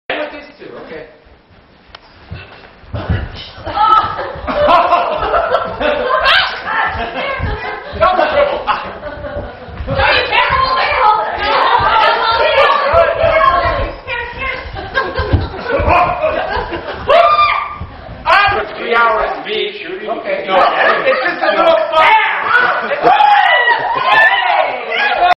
moving basket basketball
Inventing a way to play basketball with a ball you can't dribble and human baskets - at Esalen, 2006